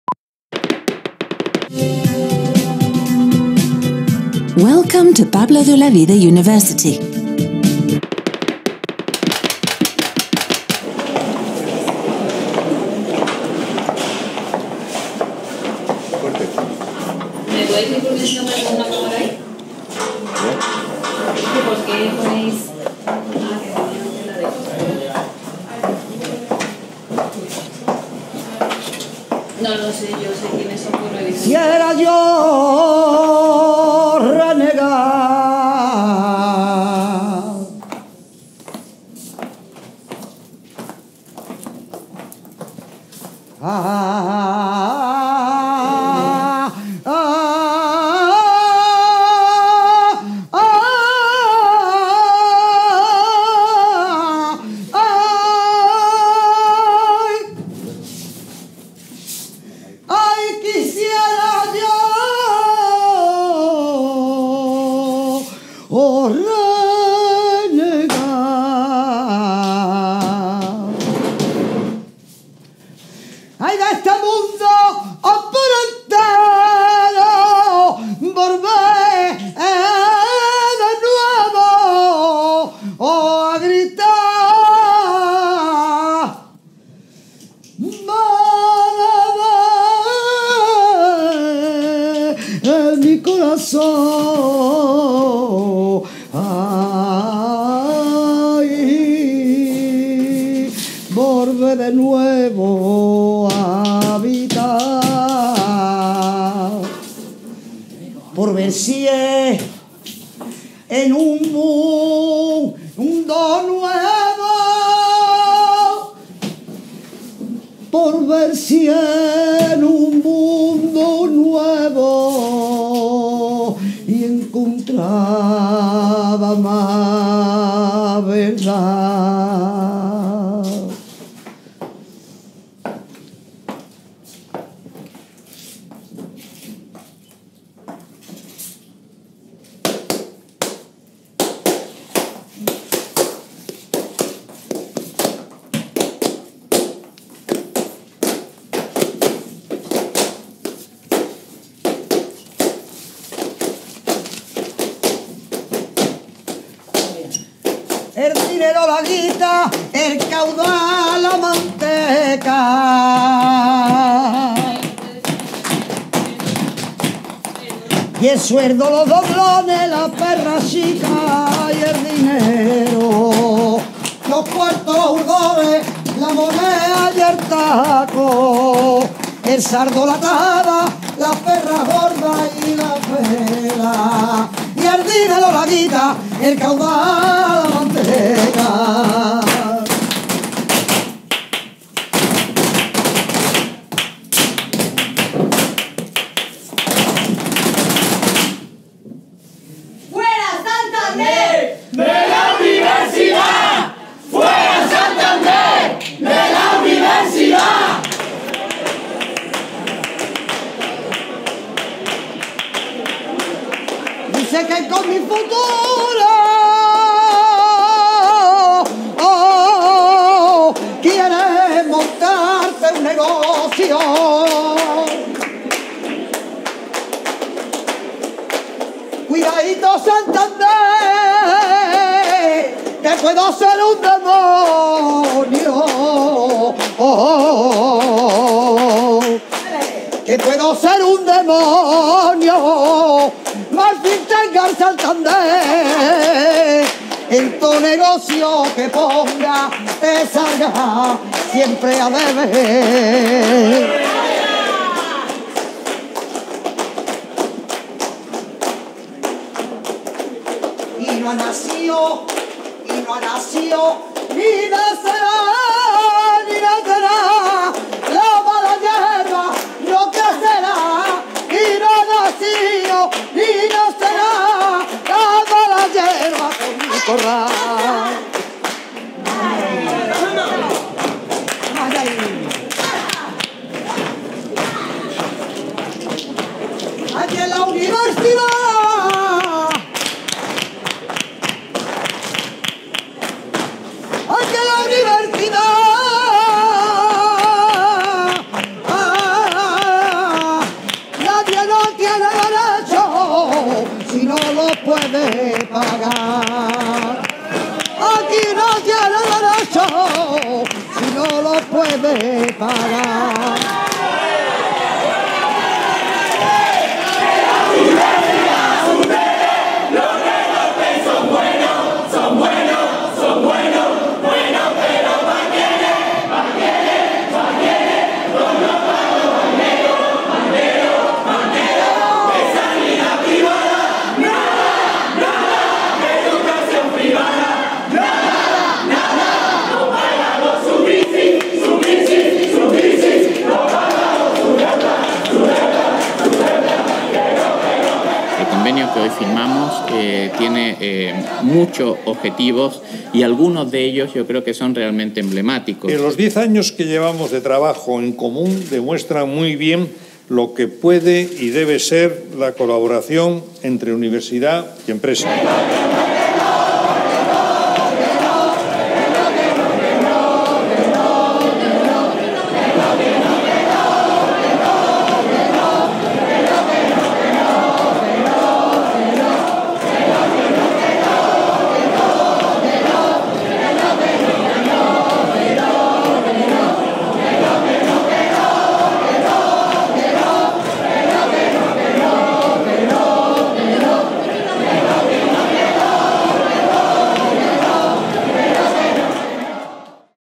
Flo6x8 - Futuro versus Usura - Univ Pablo Olavide
A las 13:00 horas del martes 25 de noviembre de 2014, tuvo lugar una acción de flo6x8 en la sucursal que el Banco Santander tiene dentro de la Universidad Pablo de Olavide (Sevilla). Colaboraron en esta acción distintos colectivos de la Universidad, entre los cuales se encontraban miembros del Consejo de Estudiantes, del Movimiento de Acción Estudiantil de Sevilla, el Grupo de Base del MAE-UPO y el Sindicato Andaluz de Trabajadores.
Mientras una masa de decenas de alumnxs y profesorxs les esperaba en la puerta de la sucursal, cuatro bailaoras, una cantaora y cuatro miembros de la comunidad universitaria, entraron en la oficina para realizar una sorpresiva acción de cante y baile donde interpretaron una pieza por peteneras y otra por soleá por bulerías. El objetivo de la misma fue la denuncia del proceso de mercantilización que está sufriendo la educación y el consiguiente lucro de bancos y empresas.
Tanto el retraimiento de las diferentes instancias del Estado como el avance hacia el desmantelamiento de la educación pública universitaria amparándose en el argumentario de la austeridad y de la crisis económica, esconden una realidad harto controvertida: el lucro de las grandes corporaciones. Mientras se manda al paro a decenas de profesorxs, se precarizan las condiciones laborales de lxs docentes, se amplía el número de alumnxs en las aulas, se aumentan las tasas, se reducen las becas, entre otras lindezas... estas entidades colonizan, parasitan y subyugan lo público con la complicidad del gobierno de la propia Universidad.
Se trata de la privatización del Estado, en este caso de la universidad pública, para beneficio de lobbies y corporaciones como la criticada en este caso, la gran beneficiaria de esta operación encubierta de privatización: el banco Santander, una de las grandes corporaciones bancarias del entorno europeo, la mayor de España y una de las más agresivas del ámbito latinoamericano. Por su gran capacidad de presión sobre el gobierno español, el Santander es responsable del rescate de la banca con fondos del Estado y de los programas de ajuste que sufre nuestro país, de la retirada de becas y de los planes que dificultan la conciliación de la vida estudiantil con la laboral, así como de todo tipo de atropellos sobre lxs estudiantes que les dificultan sufragarse los estudios por otra vía que no provenga de los recursos propios: o eres de familia pudiente o te quedas sin estudios.
El objetivo último es empobrecer a lxs estudiantes de modo que sólo puedan realizar estudios superiores recurriendo a endeudarse durante años y siguiendo así el modelo del capitalismo norteamericano y anglosajón. Estas dinámicas de empobrecer a la población para empujarla a endeudarse, ¿te suenan? El Banco Santander, con la ayuda del gobierno central, de los gobiernos autonómicos y de las autoridades universitarias (sucesivos equipos rectorales), busca reeditar en las universidades el paradigma de la deuda que, tras la congelación de salarios y el encarecimiento de la vida en los años 90 y en los 2000, nos precipitó a la crisis económica que sufrimos hoy.
No cejaremos las acciones hasta que en la Universidad Pablo de Olavide y en el resto de universidades públicas se revoquen los acuerdos con el Banco Santander y se enmienden las situaciones de precarización de lxs profesionales, así como aquellas que empujan a endeudarse a lxs estudiantes como única alternativa para disfrutar de estudios universitarios.
Por una universidad pública democrática y de calidad
La creciente cantidad de colaboradorxs que formamos parte de flo6x8 agradecemos mucho los aportes externos de estudiantes y profes de la UPO, quienes habéis hecho posible esta acción, así como la información contextual y conocimiento táctico del medio, fruto del buen hacer de las luchas democráticas en el entorno universitario.
Flo6x8
1 de diciembre de 2014, Sevilla

Letras flamencas:

Quisiera yo renegar
de este mundo por entero
volver de nuevo a habitar
mare de mi corazón
por ver si en un mundo nuevo
encontraba más verdad

El dinero la guita
el caudal la manteca
el sueldo los doblones
la perra chica y el dinero
los cuartos los urdores
la monea y el taco
el saldo la tajada
las perras gordas y las pelas

Dicen que con mi futuro
quieres montarte un negocio
cuidaito Santander
que puedo ser un demonio

Mal fin tenga el Santander
en to el negocio que pongas
te salga siempre a deber

Y no ha nacío ni nacerá
la mala hierba
no crecerá
por mi corral

Aquí en la universidad
nadie tiene derechos
si no los puede pagar